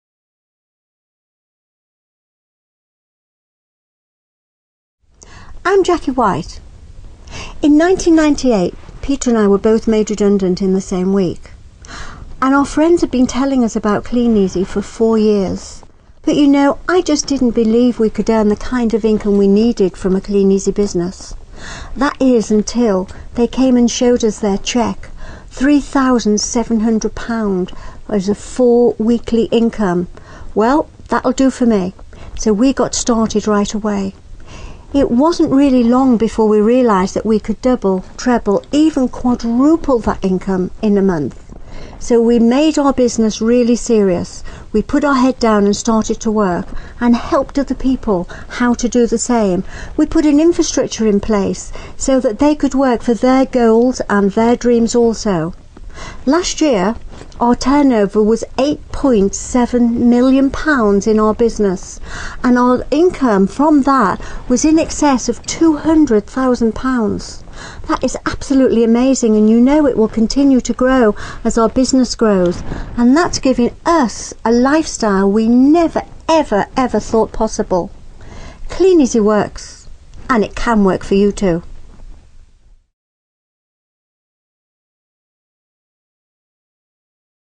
10 It works to give me the lifestyle I always dreamed of
Jackie's story: What would you do if you and your partner were both made redundant from your well-paid jobs within the same week? Faced with exactly that situation, Jackie and Peter White threw themselves into making Kleeneze work. Fourteen years on they have an enviable income of over £200k a year and a lifestyle that, back then, felt a million miles beyond their reach. If you are not currently a Kleeneze distributor and you would link FREE information on our business, please for a free information pack